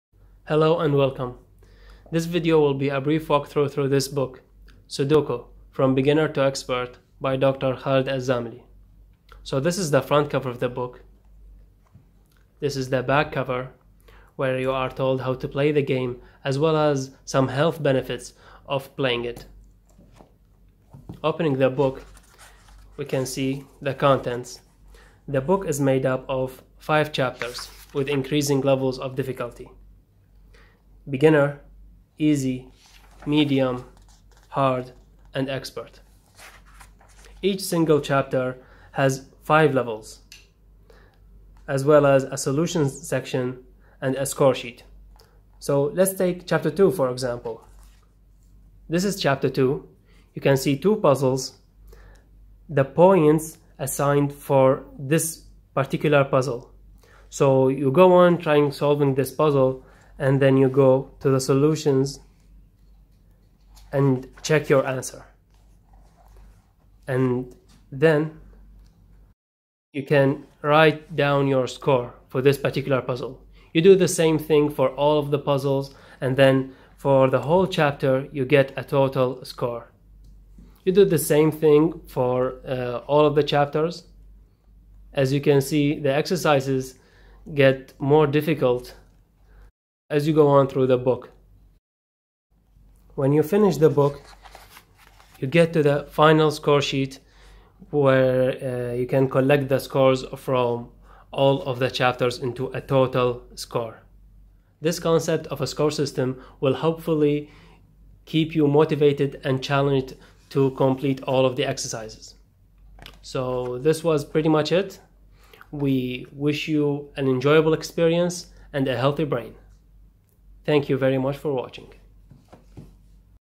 Sudoku from Beginner to Expert: 300 Puzzles in 25 Levels
The objective of Sudoku is to fill every row, column and box (3x3grid) with numbers 1-9 and each row, column, and box must have each number exactly once.
Playing Sudoku is not just a fun way to pass the time, due to its logical elements it has been found as a proven method of exercising and stimulating portions of your brain, training it even, if you will and just like training any other muscle regularly you can expect to see an improvement in cognitive functions. Some studies go as far as indicating regular puzzles can even help reduce the risk of Alzheimer's and other health problems in later life.

Completing Sudoku regularly helps the brain process both problem solving and improves logical thought process with the use of deductive reasoning, which can also be applied to approaching real life challenges in a different manner.

Playing Sudoku on a daily basis helps to keep an active mind and players often see improvements in their overall concentration.

Sudoku is an excellent game of reasoning and doesn't require any calculating nor understanding of mathematics and is therefore suitable for beginners as well as advanced players, all you require is some concentration and thought. If for gentle brain exercise or relaxing try your hand and have some fun with Sudoku.

This book has been divided into five chapters ranging in level from beginner, easy, medium, hard until expert.

Each puzzle can earn you between 1 and 25 points according to difficulty. There is a score sheet at the end of each chapter where you can fill in your score. When you have worked through each chapter, add up your points and see how close you are to the maximum possible. At the end there is a sheet where you can total your points for the whole book. How close can you get to the perfect score of 3900 points?

Whatever score you achieve, we hope this will be an entertaining and uplifting mental workout, enjoy Sudoku from beginner to expert.